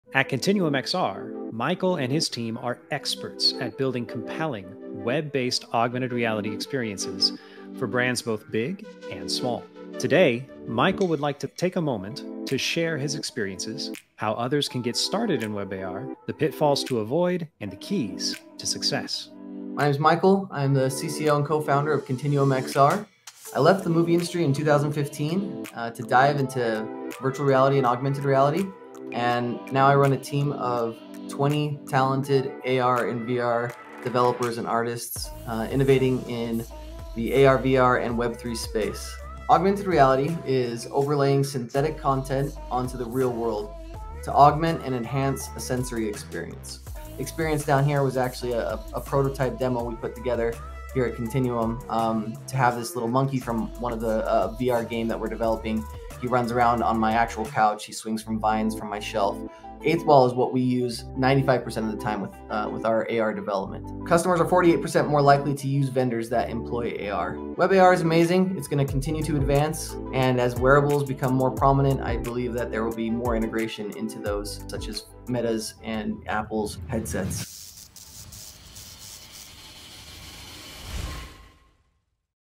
The future of Augmented Reality and what it means for you
Join us as we get together to hear from Continuum’s CEO and Co-Founder, Michael Murdock. Michael gives us great insights into the future of Augmented Reality and what it means for you.